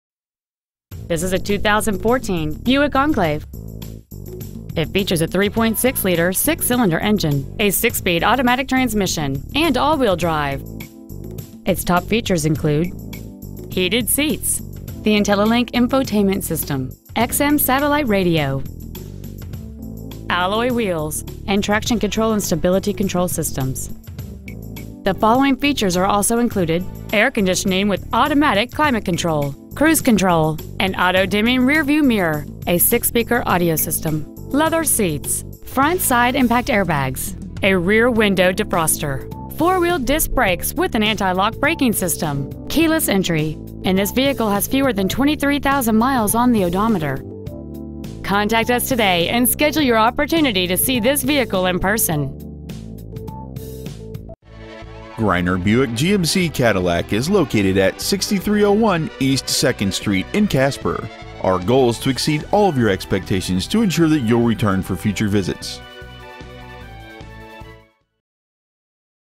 Used 2014 Buick Enclave Casper WY
We have been honored to serve the Casper WY area , we promise that your experience at our dealership will exceed your expectations ! Year : 2014 Make : Buick Model : Enclave Engine : 3.6l v6 sidi dohc vvt Trans . : 6-speed automatic Exterior : white Miles : 22,210 Interior : white Stock : UP5806 Greiner Buick GMC Cadillac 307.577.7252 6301 E 2nd St. Casper , WY 82609